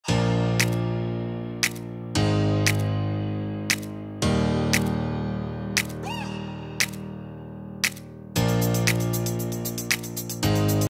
Hell's Gamer Intro made
This is the intro made by me for a new Gaming Chanel made by my brother.